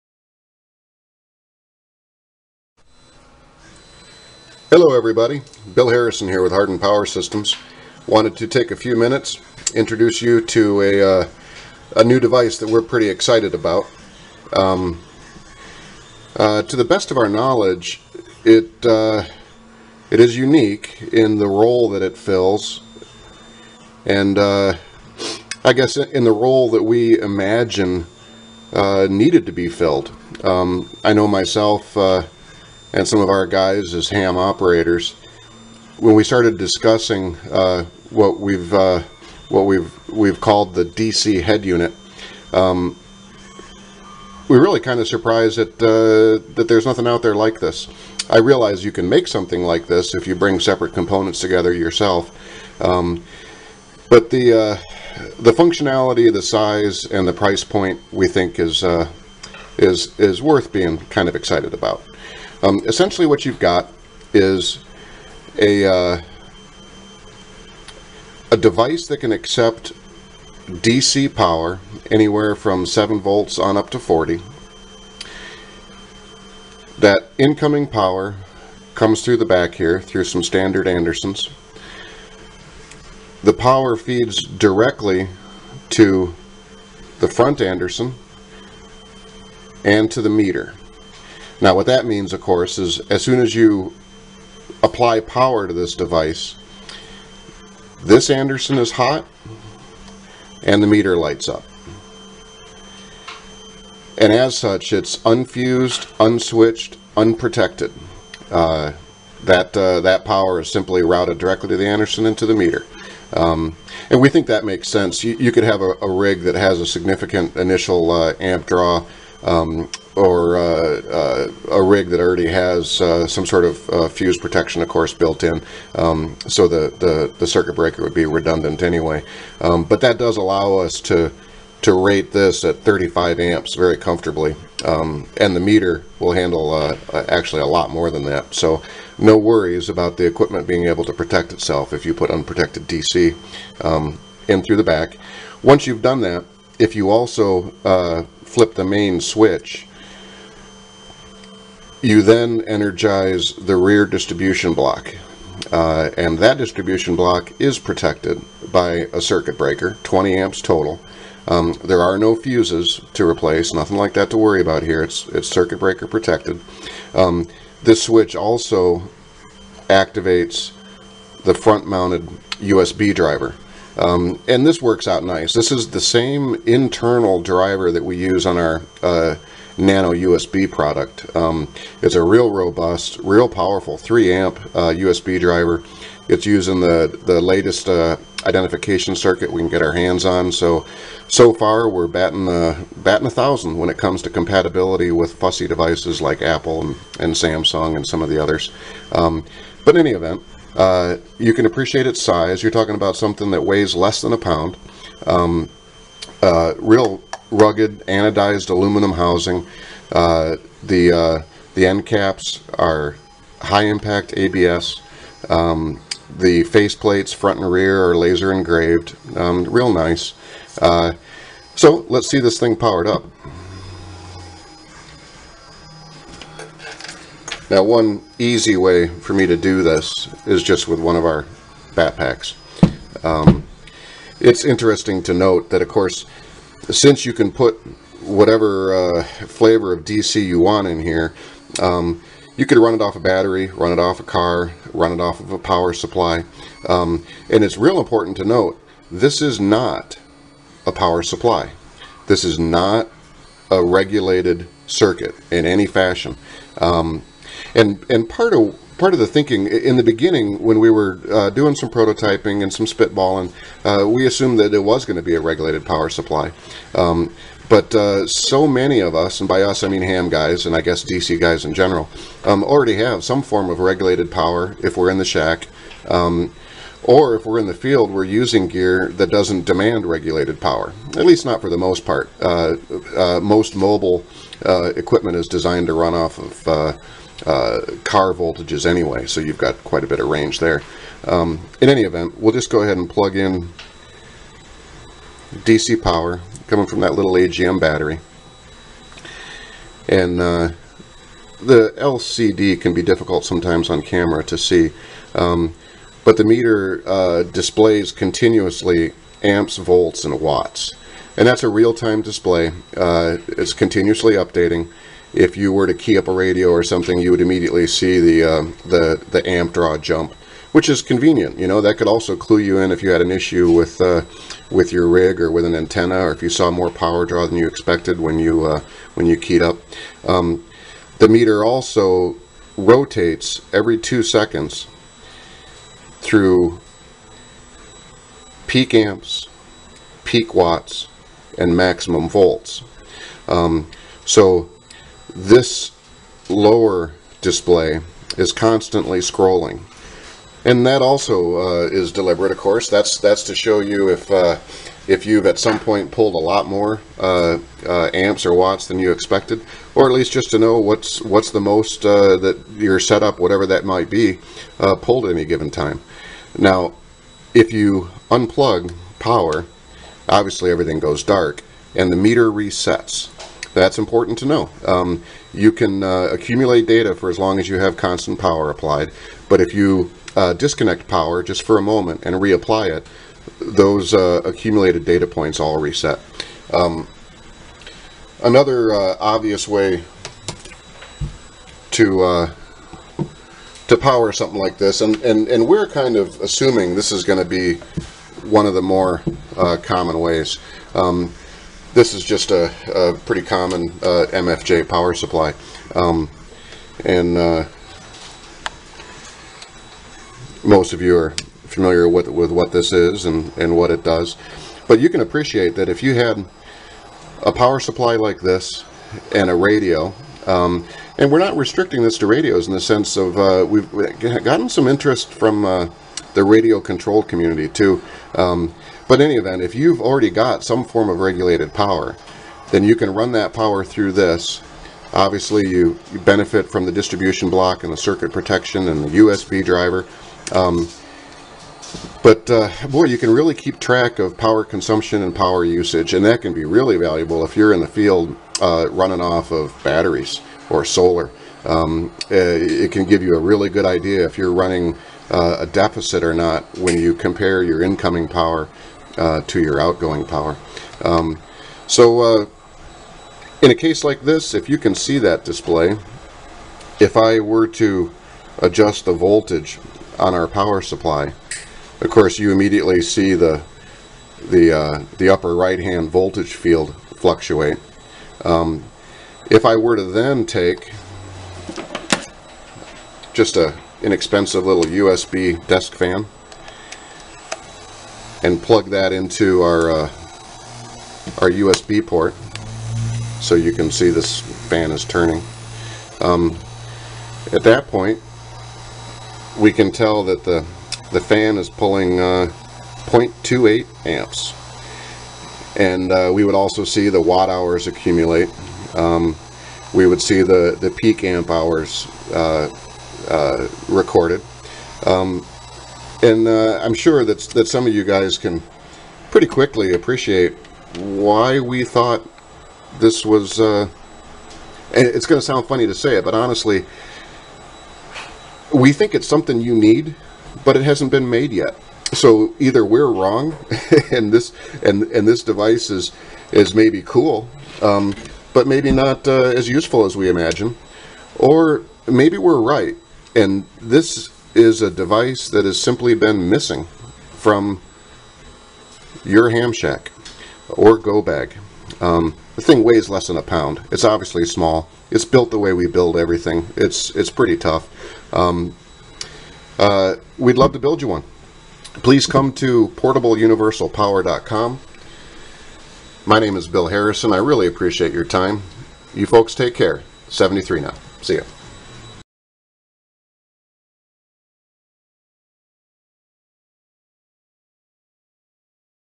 Introduction to the DC Head Unit
Our DC Head Unit is very useful component for DC applications.  It protects your gear, monitors power usage, distributes multiple connections, and does it all in a simple, rugged, package.